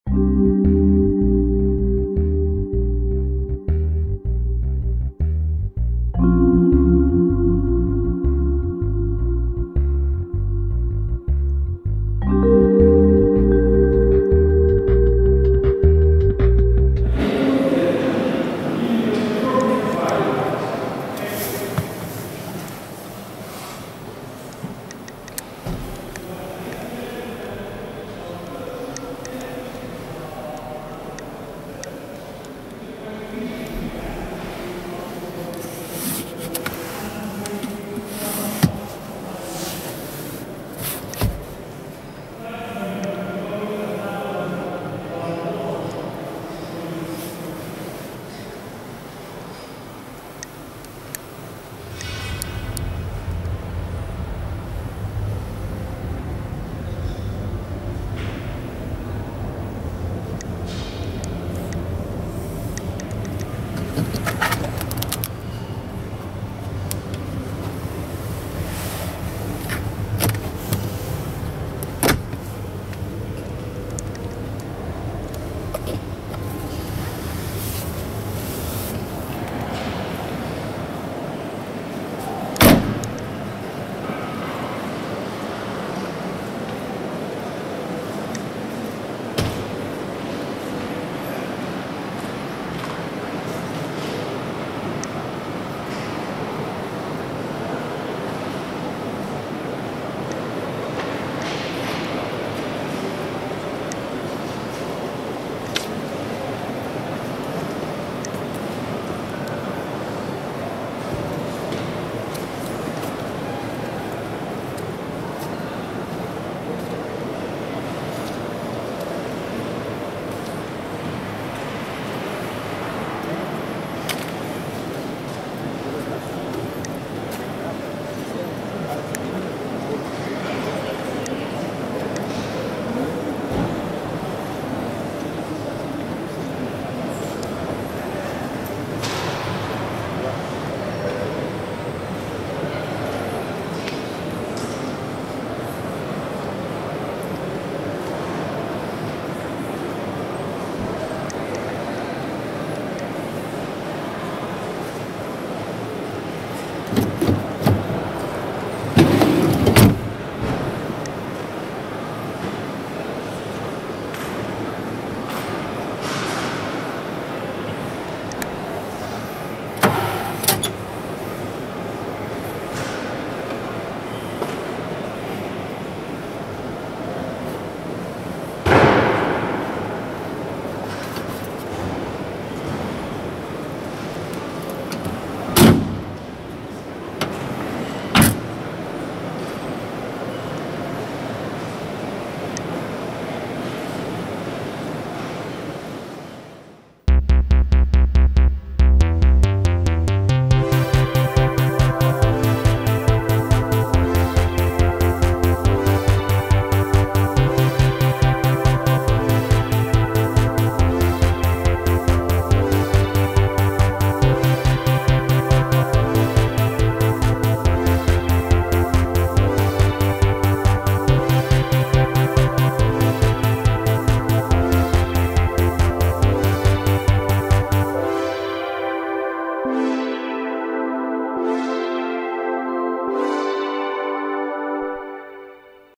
Citroën Berlingo Panel Van L2 e-HDi Niveau B 4x4 Exterior and Interior
Citroen Berlingo Panel Van L2 e-HDi 90 Stop&Start Niveau B 4x4 Vermessungsfahrzeug seen from outside and inside. Color is Aluminiumgrau metallic (Aluminiumgrey metallic). Price in Germany starts from 17 200 EUR. Shown car costs 36727 EUR. World debut.

The van was presented at 2014 IAA commercial vehicles, Hannover, Germany - Thursday, 25 September - Thursday, 2 October 2014.